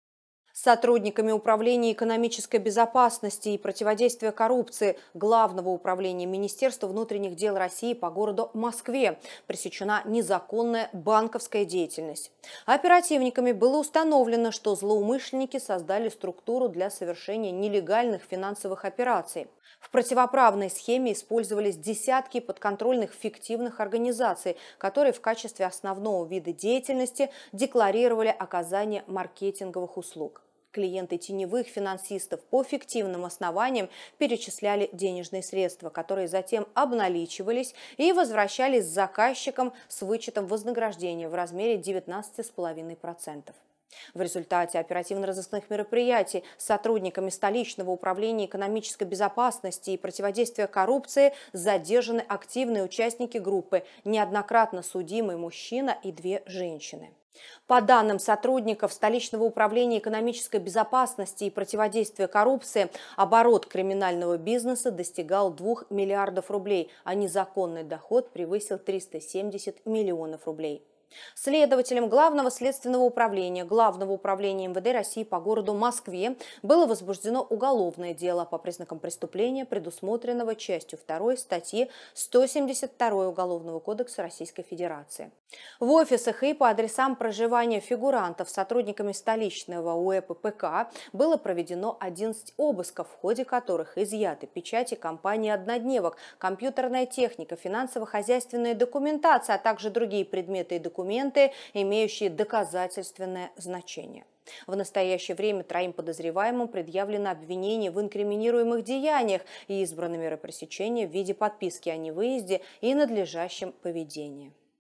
Ирина Волк: В Москве задержаны подозреваемые в незаконной банковской деятельности
Сотрудниками Управления экономической безопасности и противодействия коррупции ГУ МВД России по г. Москве пресечена незаконная банковская деятельность.

«Оперативниками установлено, что злоумышленники создали структуру для совершения нелегальных финансовых операций. В противоправной схеме использовались десятки подконтрольных фиктивных организаций, которые в качестве основного вида деятельности декларировали оказание маркетинговых услуг. Клиенты теневых финансистов по фиктивным основаниям перечисляли денежные средства, которые затем обналичивались и возвращались заказчикам с вычетом вознаграждения в размере 19,5 %.

В результате оперативно-розыскных мероприятий задержаны активные участники группы – неоднократно судимый мужчина и две женщины», - сообщила официальный представитель МВД России Ирина Волк.

По предварительным данным, оборот криминального бизнеса достигал двух миллиардов рублей, а незаконный доход превысил 370 миллионов рублей.

Следователем Главного следственного управления ГУ МВД России по городу Москве возбуждено уголовное дело по признакам преступления, предусмотренного частью второй статьи 172 УК РФ.

«В офисах и по адресам проживания фигурантов проведено 11 обысков, в ходе которых изъяты печати фирм-однодневок, компьютерная техника, финансово-хозяйственная документация, а также другие предметы и документы, имеющие доказательственное значение.

Оперативно-розыскные мероприятия и следственные действия, направленные на установление всех обстоятельств произошедшего, продолжаются.